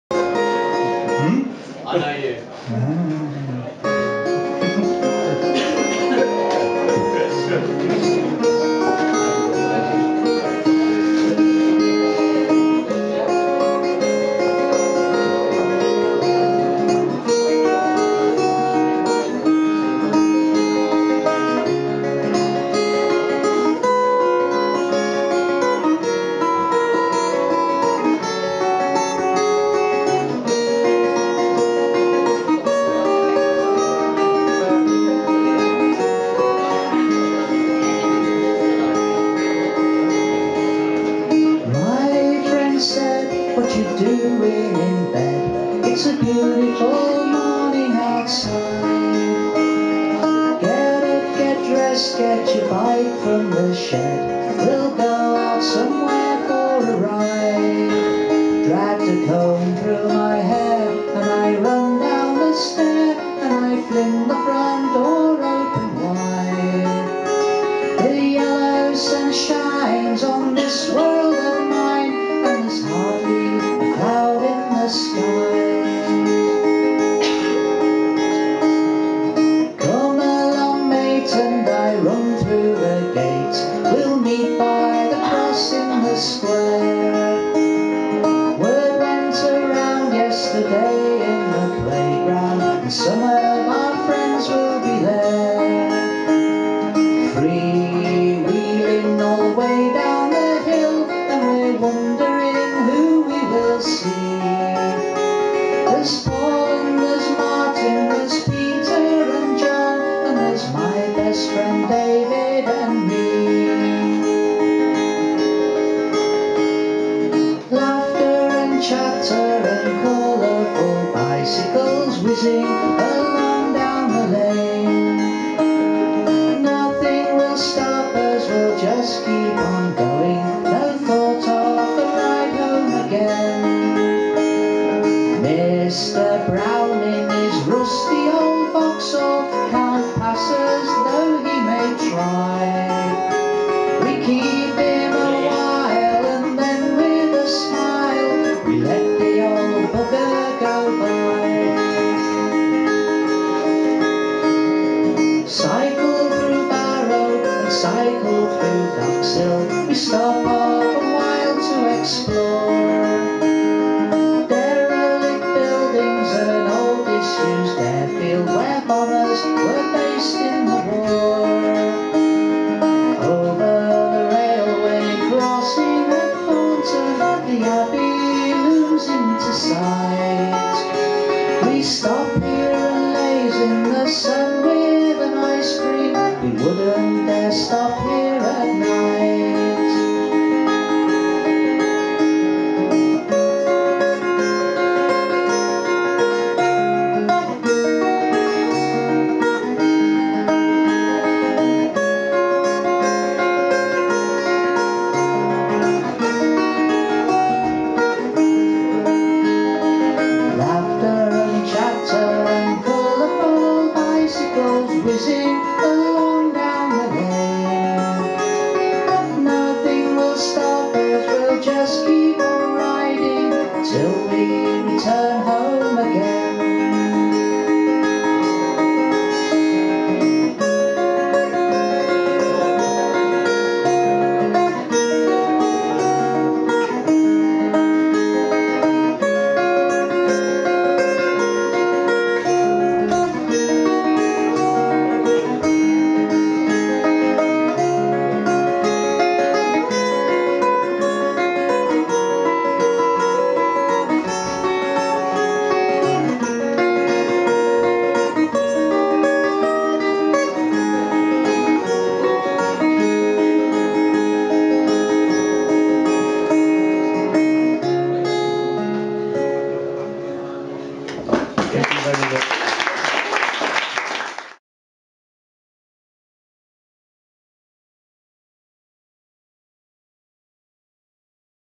Ian Bellwood at The Grove Festival Leeds Fringe 2011
Ian Bellwood live at the Grove Festival Leeds Fringe 2011